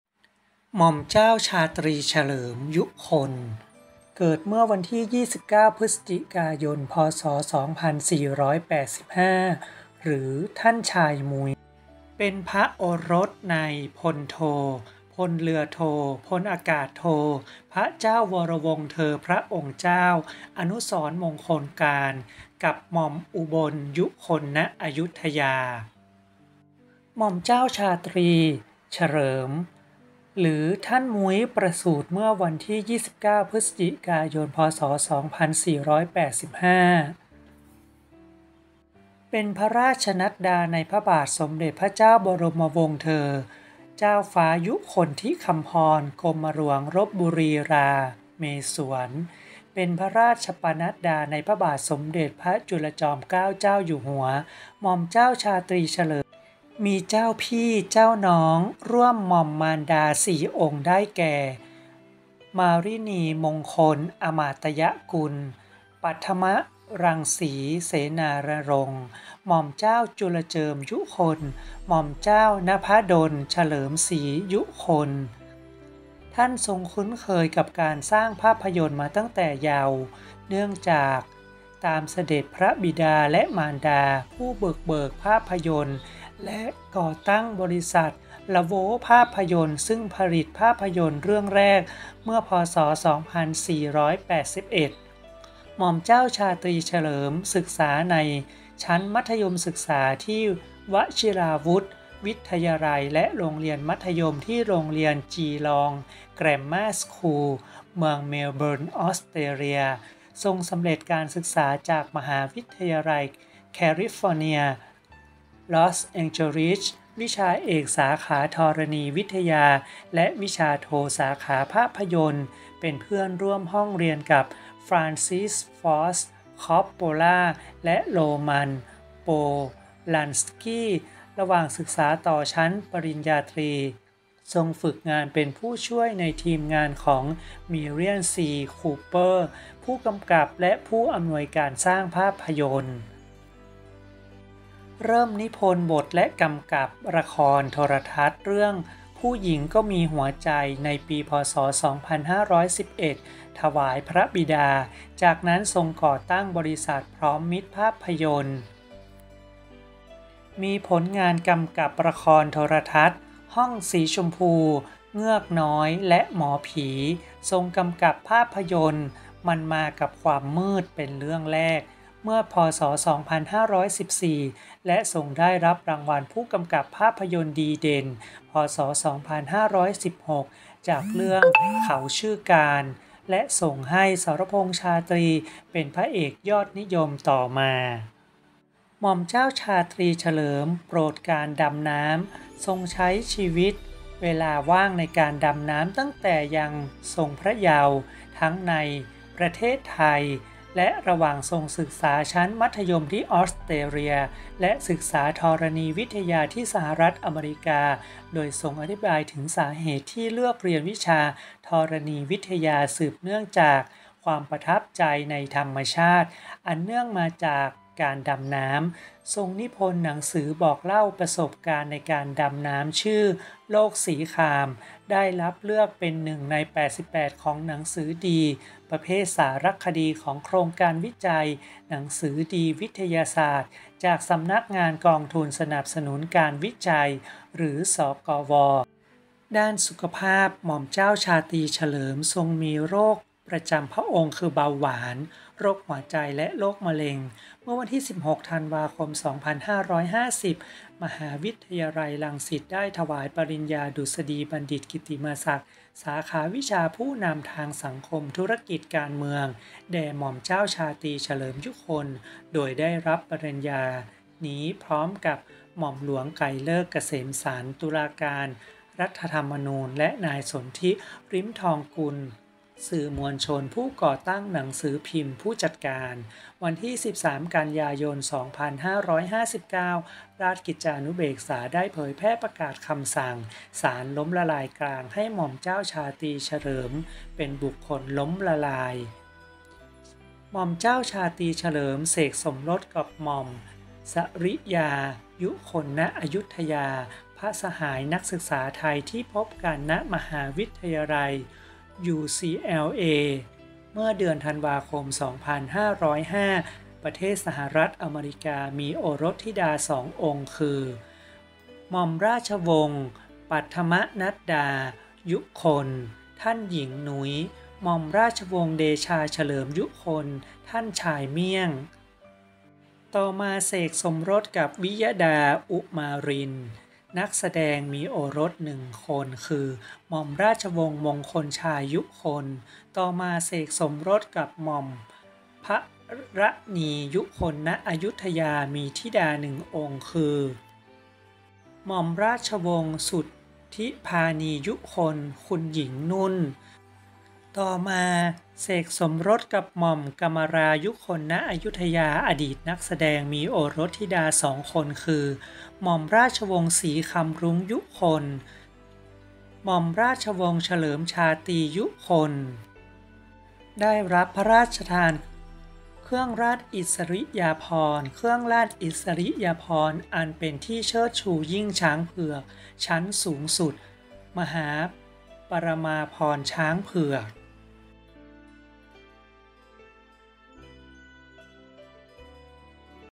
หม่อมเจ้าชาตรีเฉลิม ยุคล
ม่อมเจ้าชาตรีเฉลิม ยุคล  หรือ ท่านชายมุ้ย  เป็นพระโอรสในพระเจ้าวรวงศ์เธอ พระองค์เจ้าอนุสรมงคลการ กับหม่อมอุบล ยุคล ณ อยุธยา ถือเป็นรัชทายาทโดยสันนิษฐาน ลำดับที่ 5 ตามกฎมณเฑียรบาลว่าด้วยการสืบราชสันตติวงศ์ พระพุทธศักราช 2467